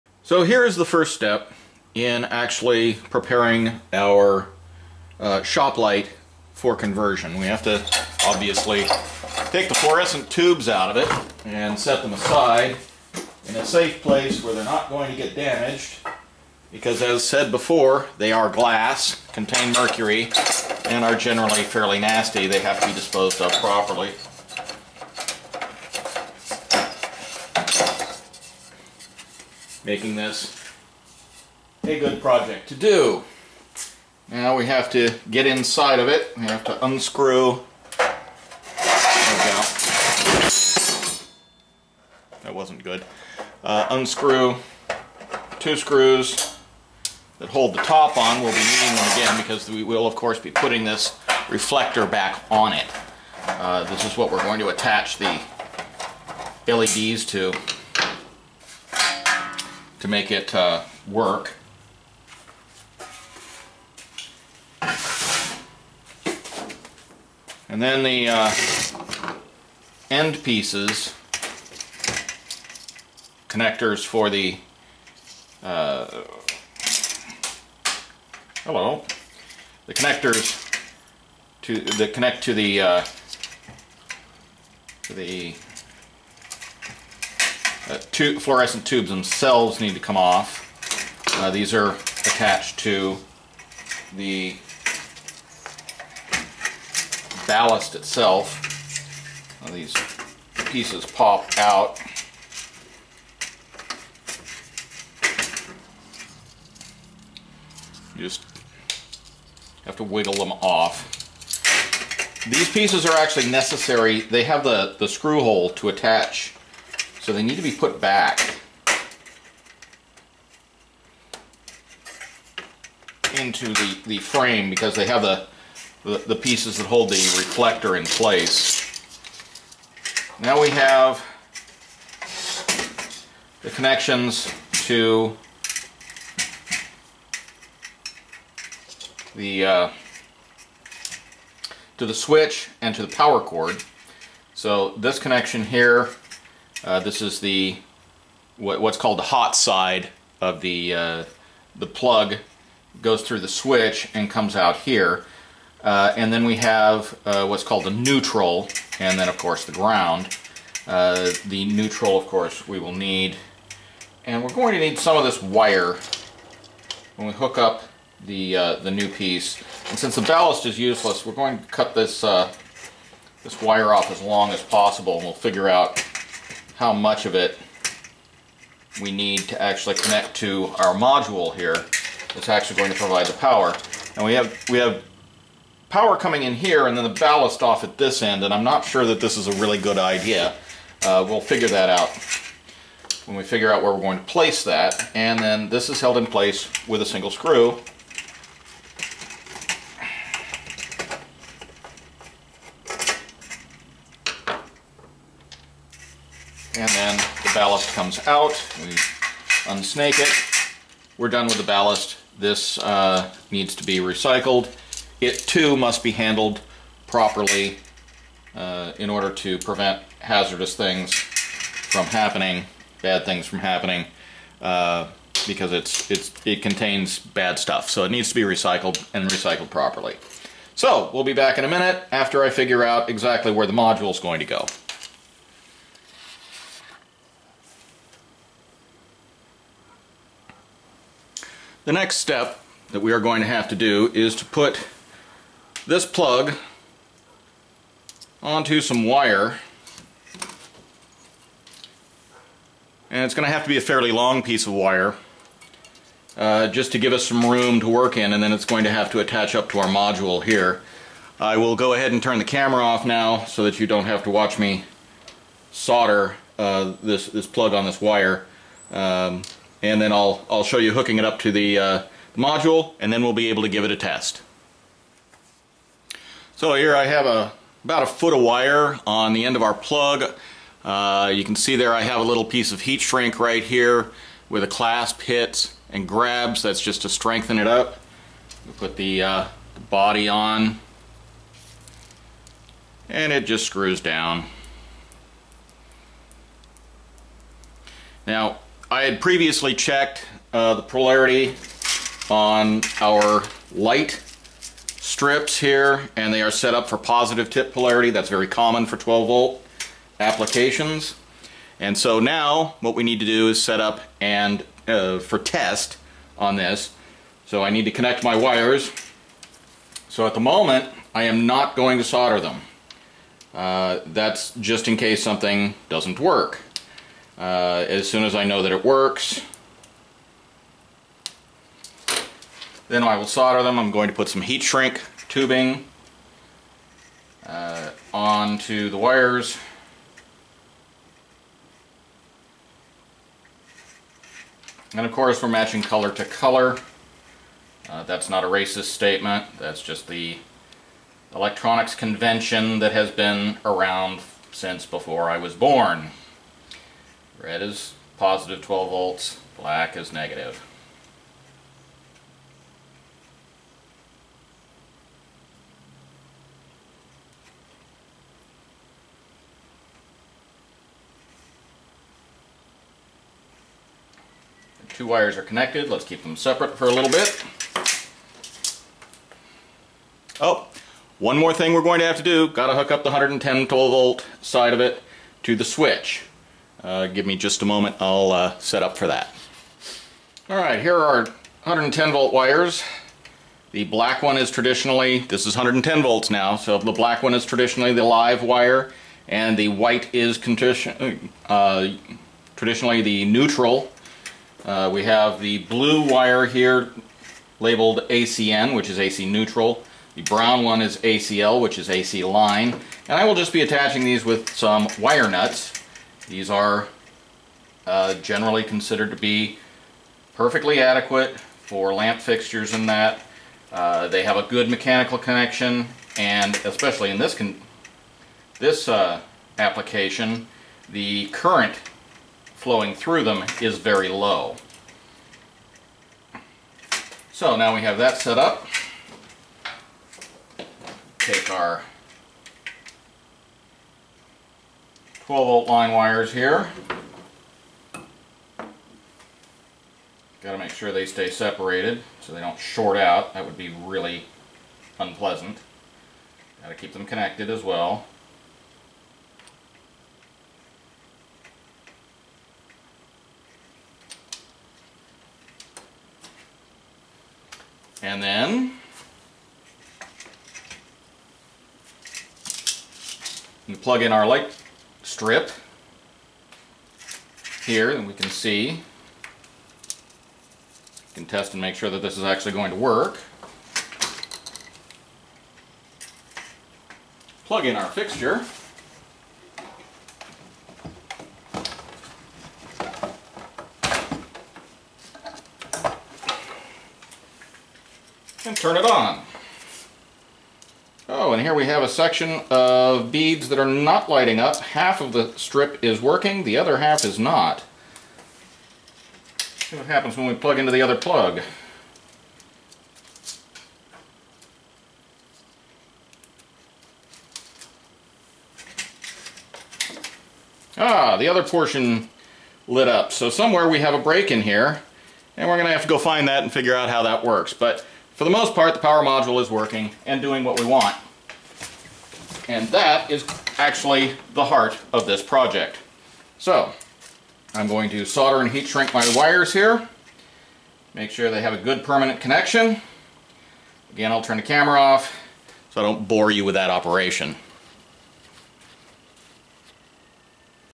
Redneck Conversion: Fluorescent shop light to LED, installment 2
I get to work on actually converting this cheap shop light to LED lighting.  Stripping, gutting and prepping for its new life.

The LED tape came from Amazon